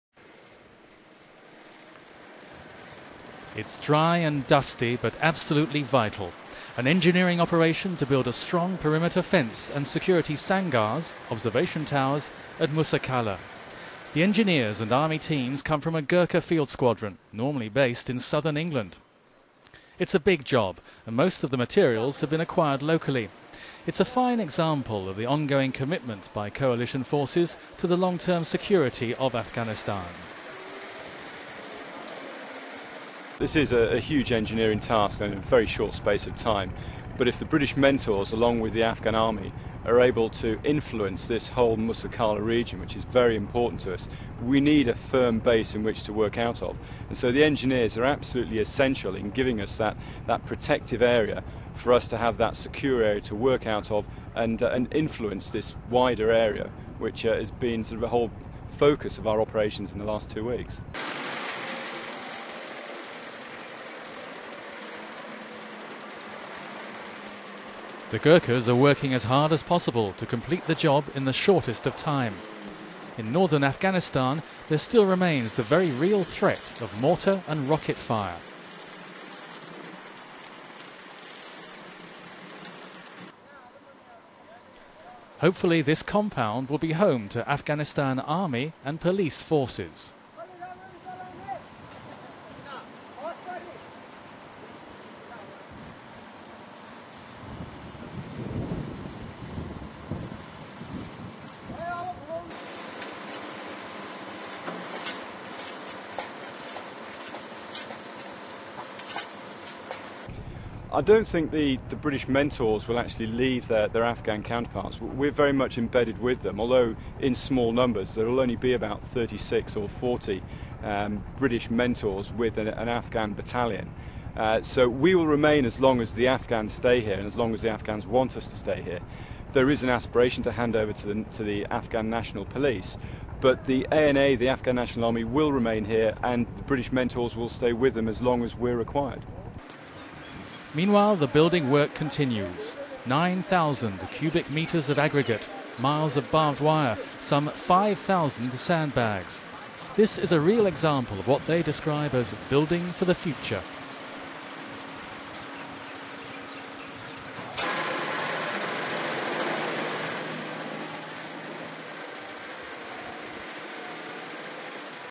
BRITISH ENGINEERS HELP IN AFGHANISTAN
British Army engineers  have been helping design, build and install security protection fences in and around a strategically important town in northern Afghanistan.